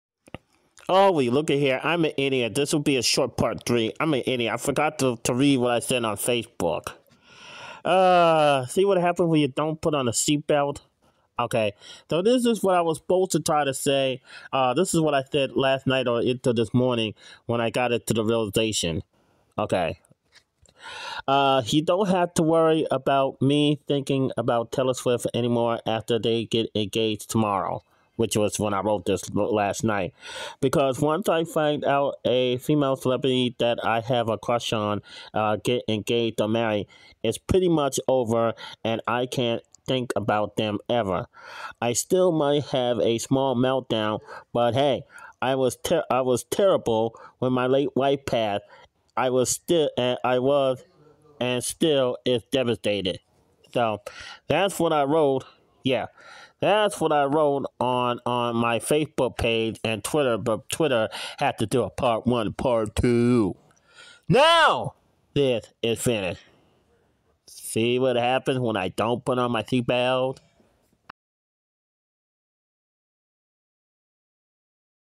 meltdown watch part three i forgot
Wow today is Taylor Swift's birthday and I know that Travis k is going to propose to her but why wait till tonight at the party ? Do it now so I can let it out by yelling and crying and having flashbacks when Dr Renee ruined my life when she told me my late wife passed away back in 2016 .  This is part three.